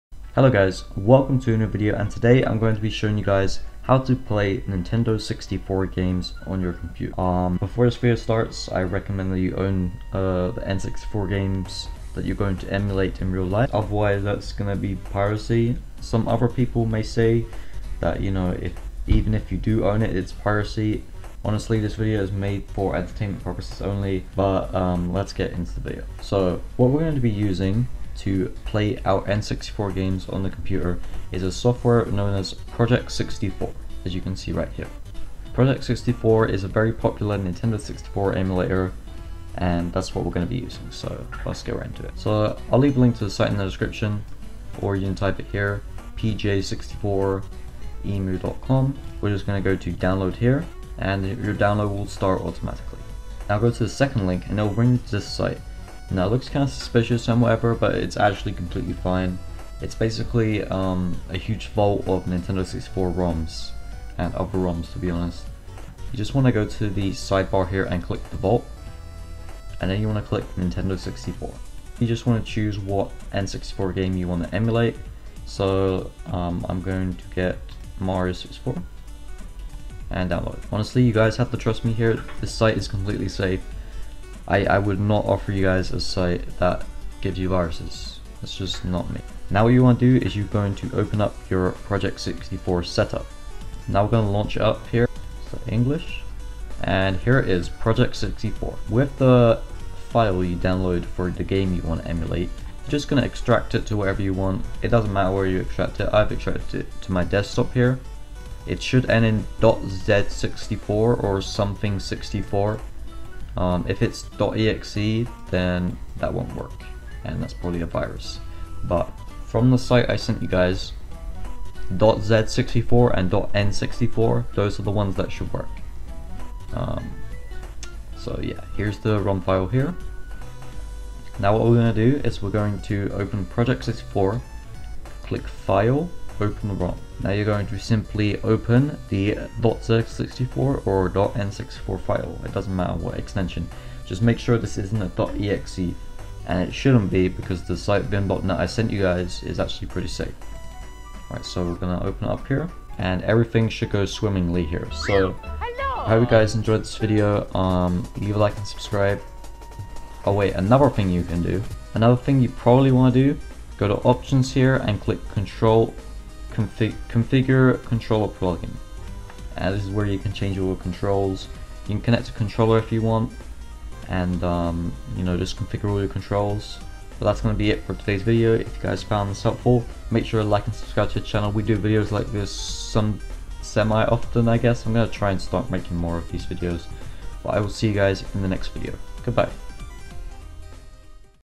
How to play N64 Games on PC! (2020)
Hello everyone, Mushy Tutorials here

Today I'll be showing you guys a very easy way on how to play/emulate your favourite nintendo 64 games on the pc. We will be using a very popular emulator called Project64 to do this, although there are alternatives like mupen64plus. Enjoy!